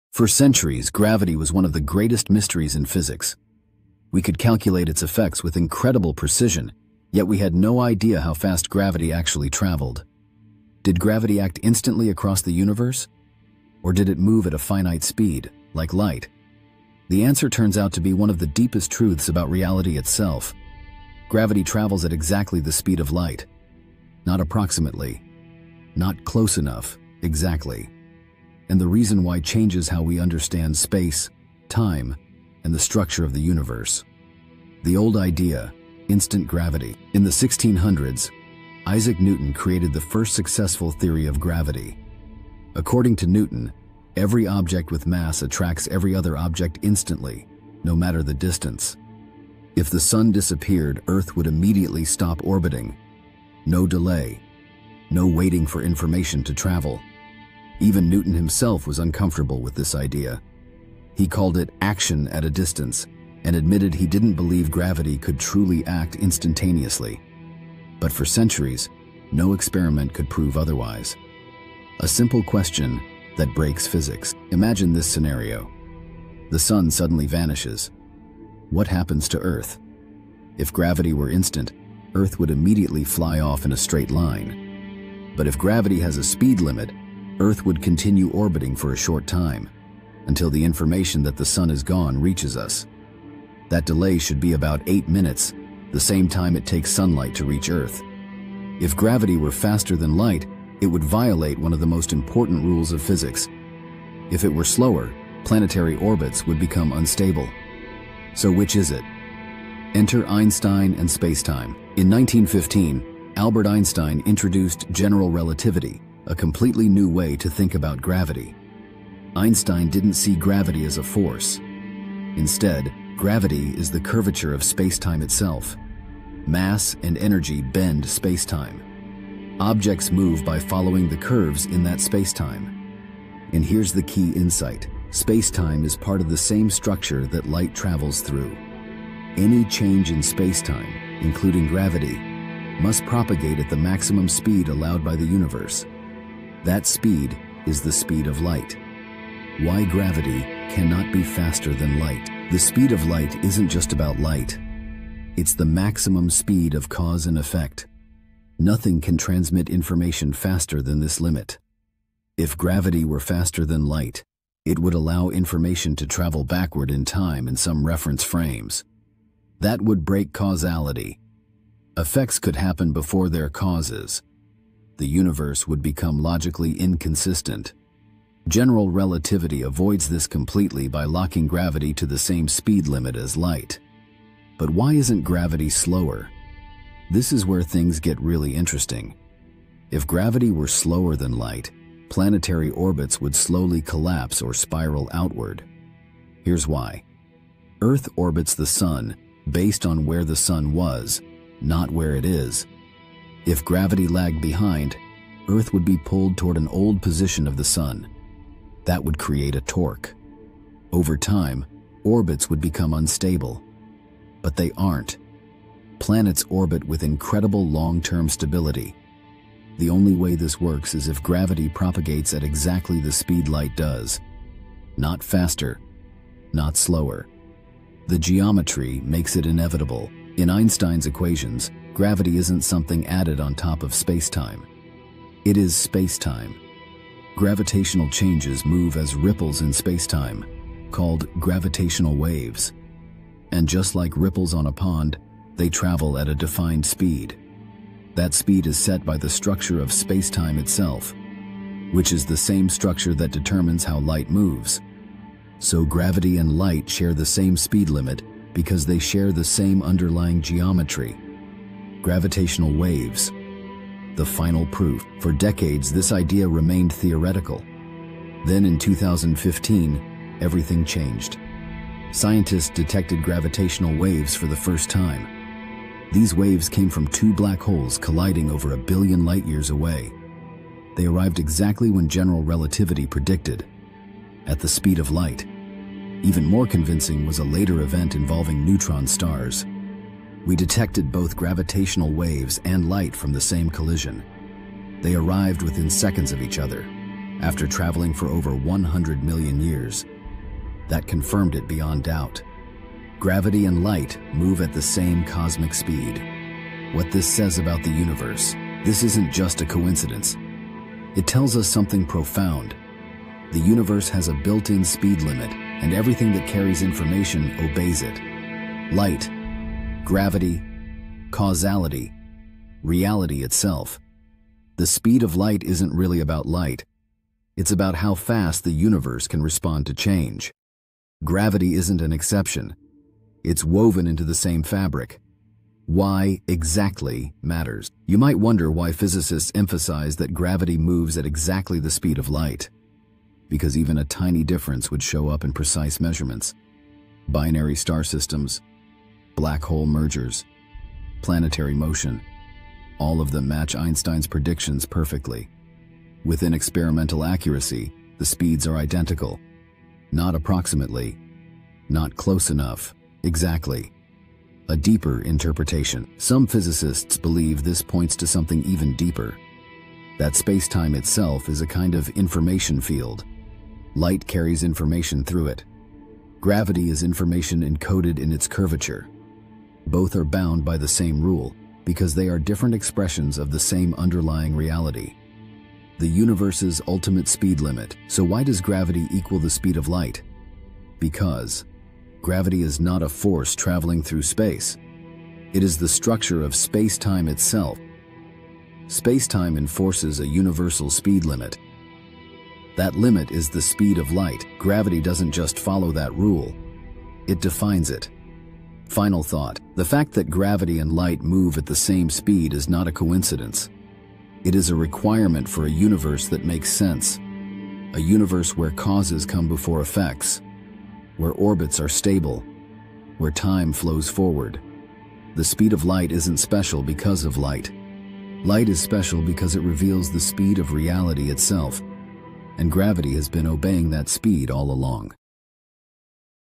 Why the Speed of Gravity Equals the Speed of Light Exactly
Why the Speed of Gravity Equals the Speed of Light Exactly | Einstein, Relativity & Physics Explained

Have you ever wondered why the speed of gravity is exactly the same as the speed of light? In this video, we dive deep into the mysteries of gravity, Einstein’s general relativity, gravitational waves, and the fundamental laws of physics that govern our universe. Learn why gravitational effects travel at light speed, how this impacts cosmic events, and what this tells us about space, time, and the fabric of the universe.

We’ll explore:

The connection between gravity and light
Albert Einstein’s theory of relativity explained in simple terms
How gravitational waves were discovered and measured
Why changes in gravity propagate at light speed across the universe
The science behind black holes, neutron stars, and cosmic collisions
Real-world implications for astrophysics, cosmology, and space exploration